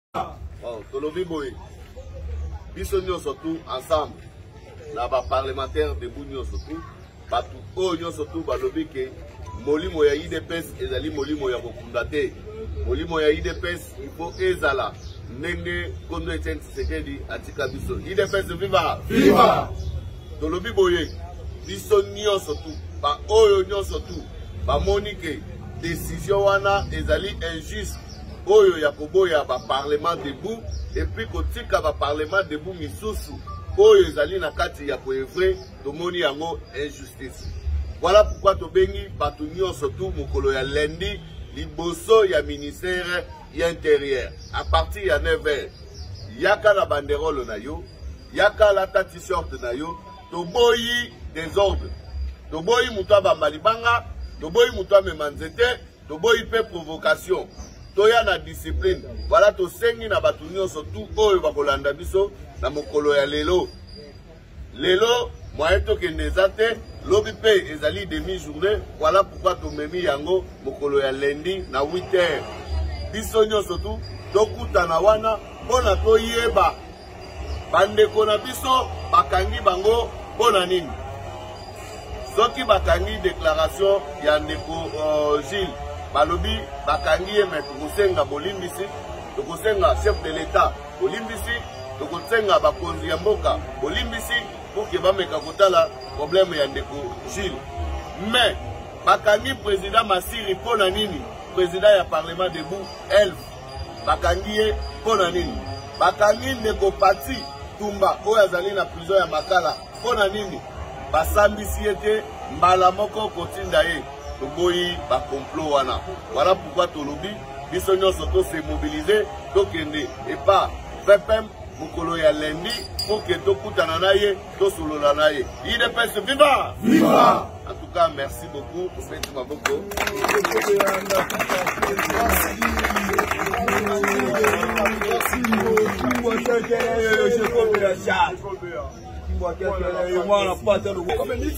DÉCLARATION YA BA PARLEMENTAIRE DÉBOUT DE L'UDPS PONA KO CONTESTER DÉCISION YA MINISTRE INTÉRIEUR
Vidéo de Directeur De Programme